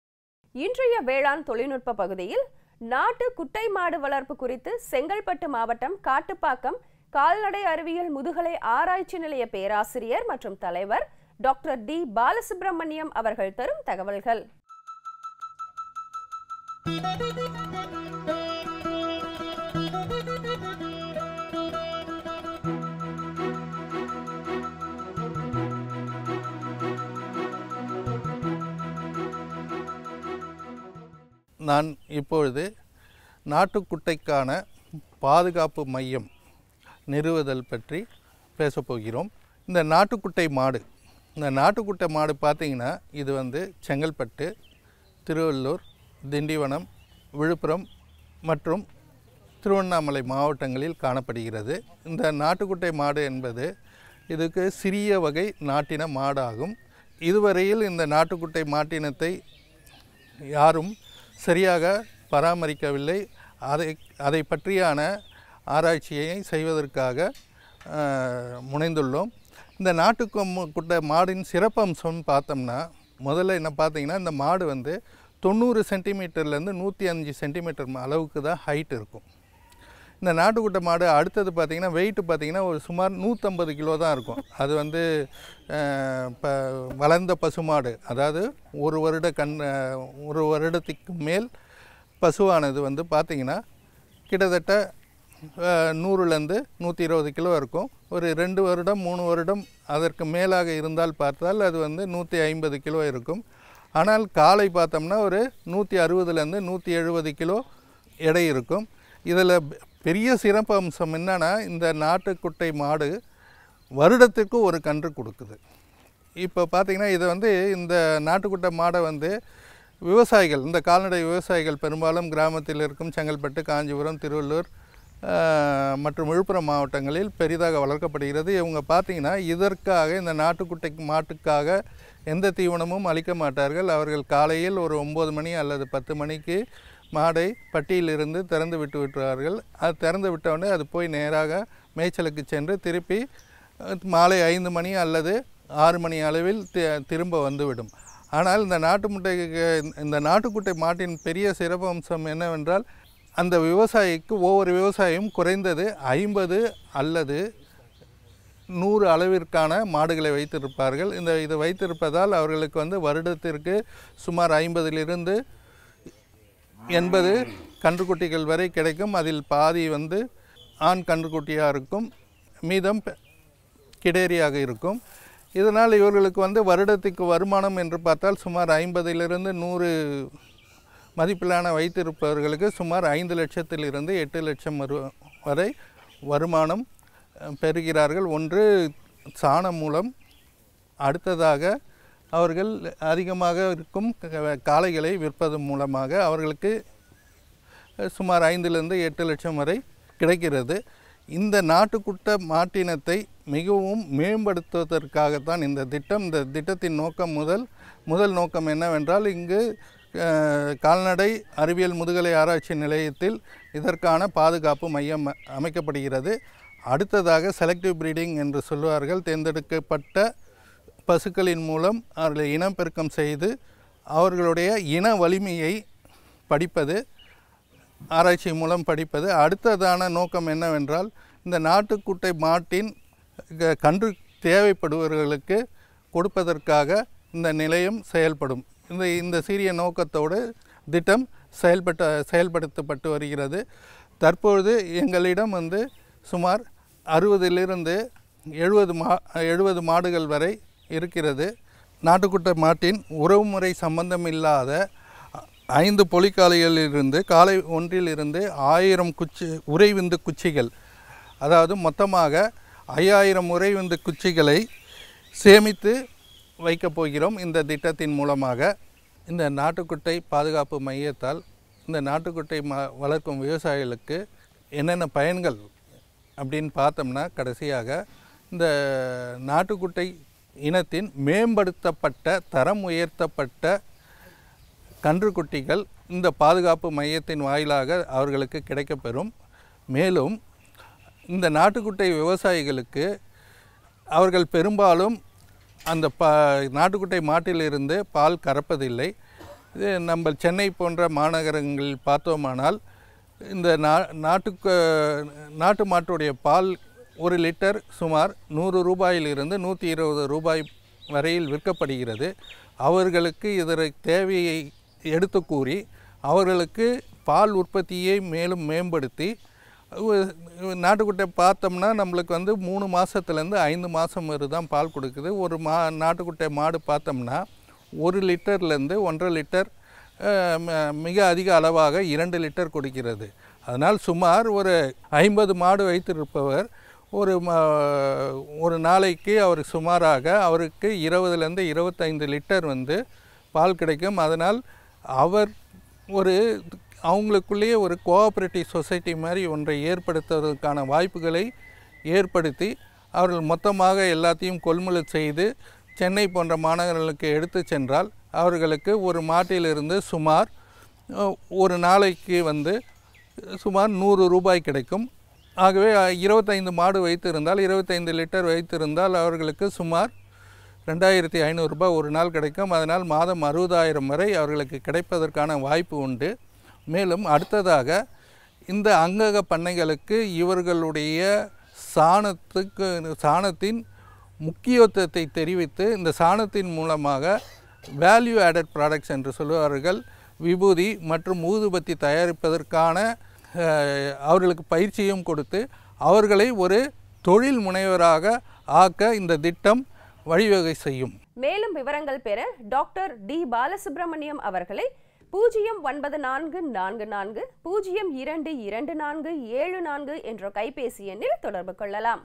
நாட்டு குட்டை மாடு வளர்ப்பு | Agricultural Technology
Domestic calf rearing

Pt: Dr. D. BALASUBRAMANYAM, Professor and Head,
Post Graduate Research Institute in Sciences,
Kattupakkam, Chengalpattu District